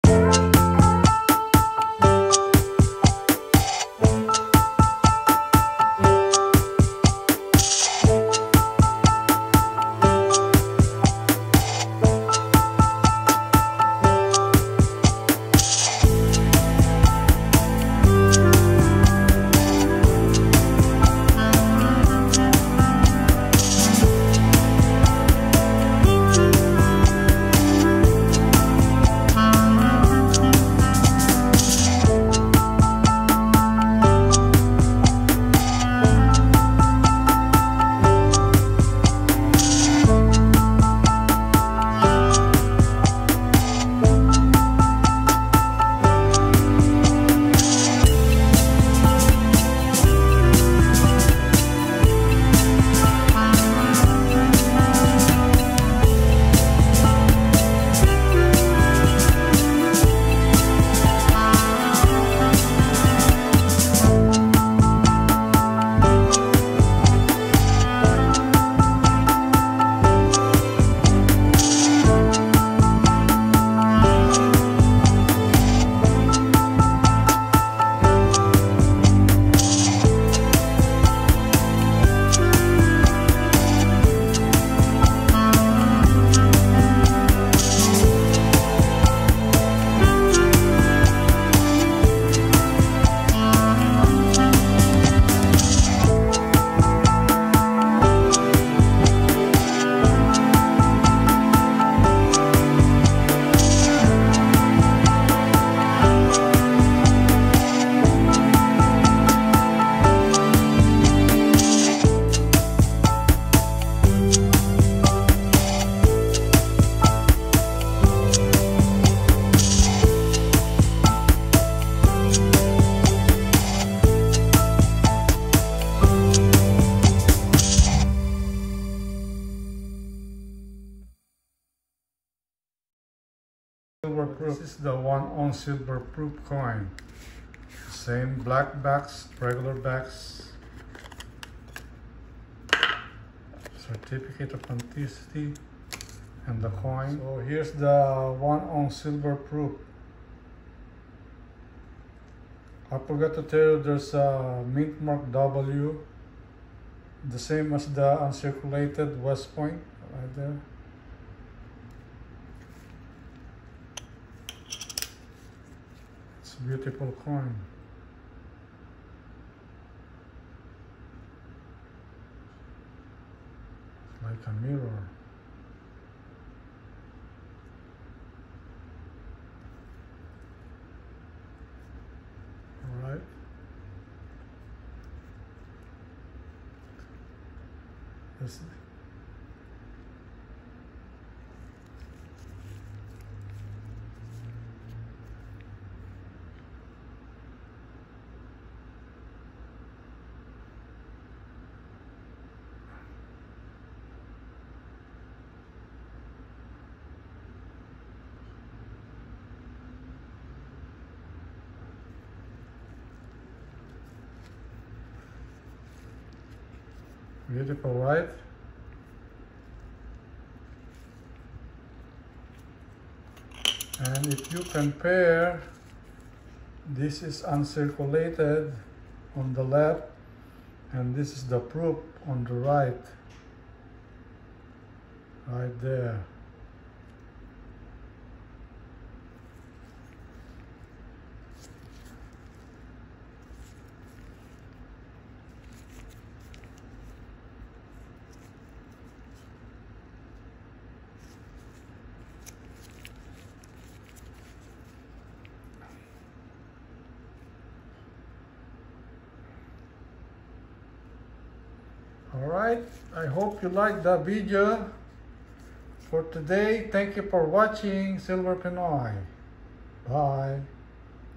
2022 1 OZ American Eagle Silver Proof coin and 2022 1 OZ Uncirculated silver coins .
The different of 1OZ  American Eagle silver proof coin and Uncirculated silver coin.